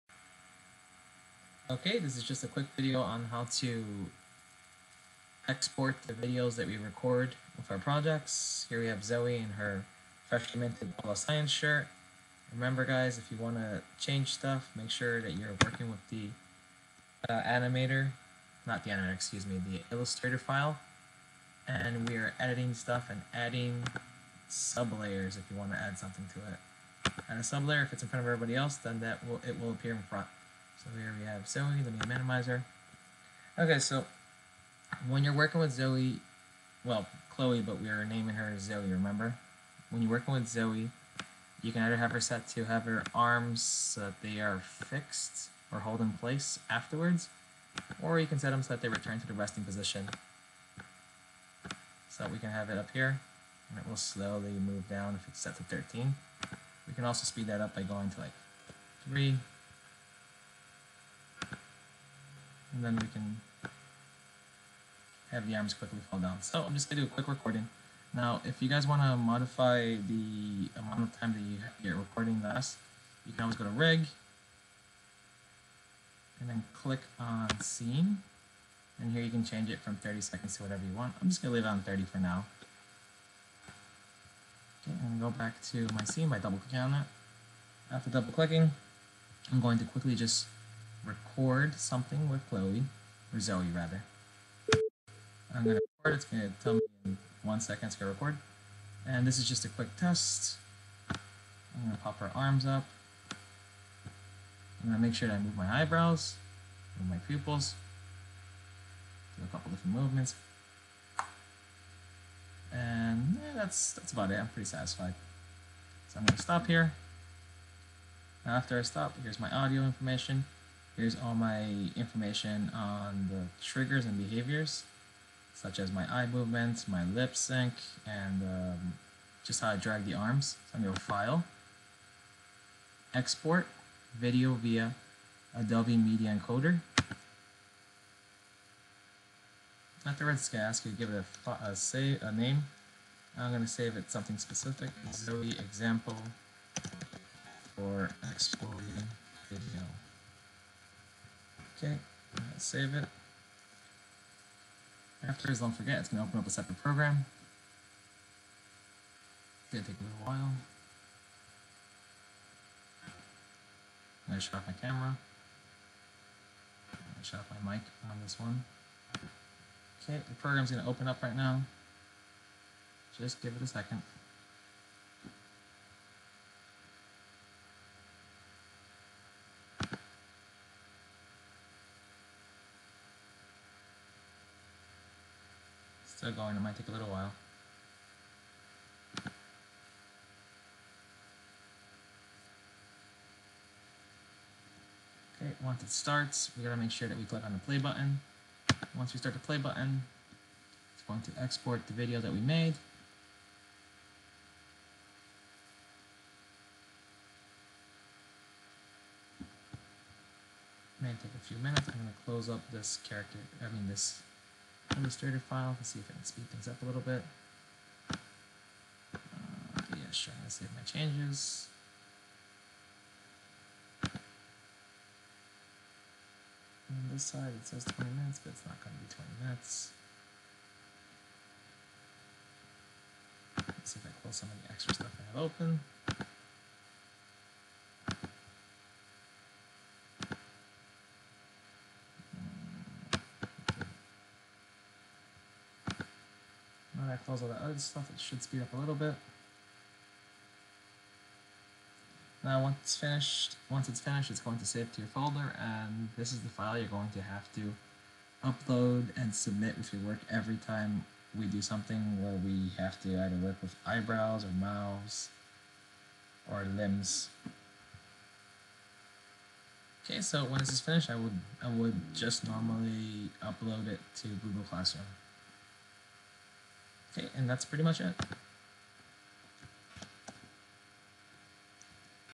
exporting a scene recording from Character Animator
The background buzzing is the computer's fan trying to keep it cool while running Character Animator along with a bunch of other programs.